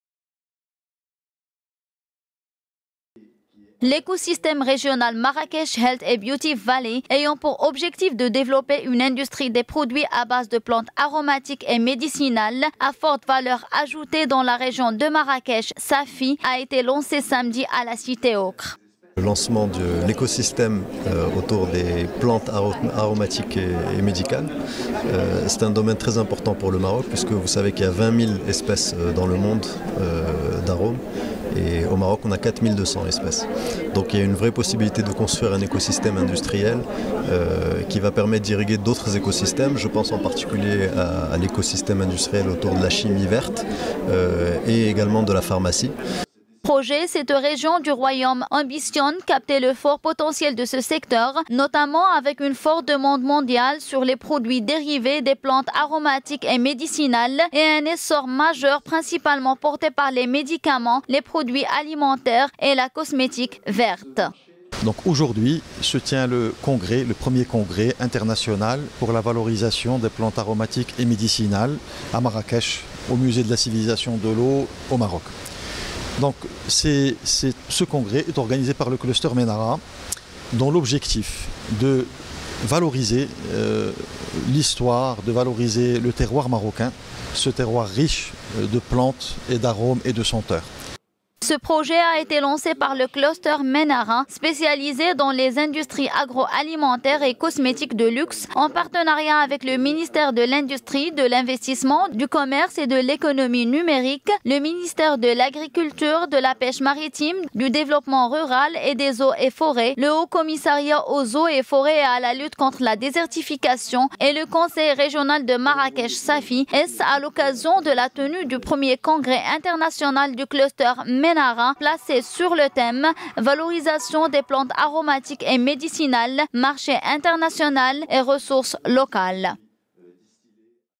Lancement de l’écosystème régional "Marrakech, Health and Beauty Valley"
L’Ecosystème régional "Marrakech, Health et Beauty Valley", ayant pour objectif de développer une industrie des produits à base de Plantes aromatiques et médicinales (PAM) à forte valeur ajoutée dans la région de Marrakech-Safi, a été lancé samedi, à la Cité ocre.